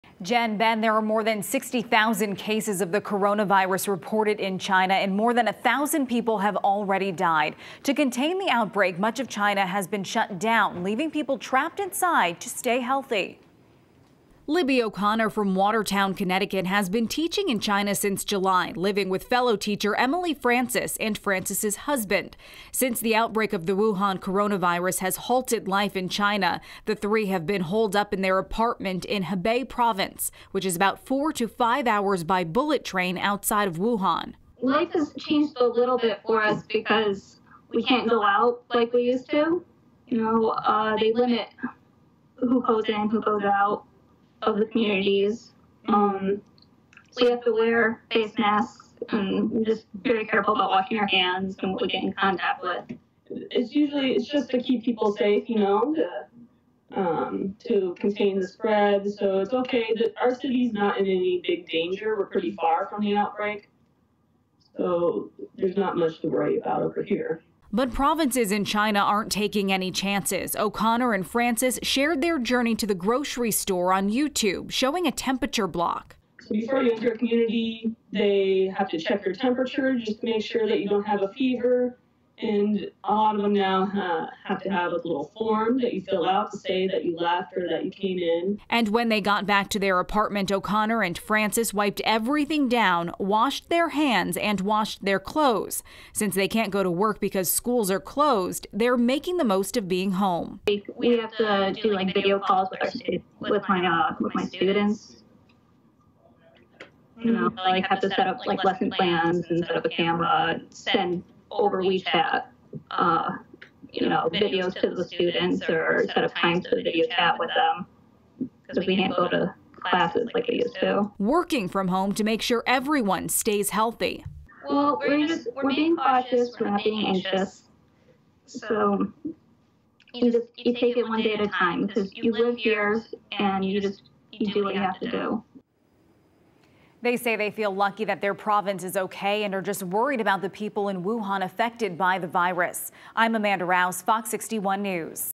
Watertown woman discusses living in China during the coronavirus outbreak
Two women discuss what its like to be stuck inside their apartment during the coronavirus outbreak in China.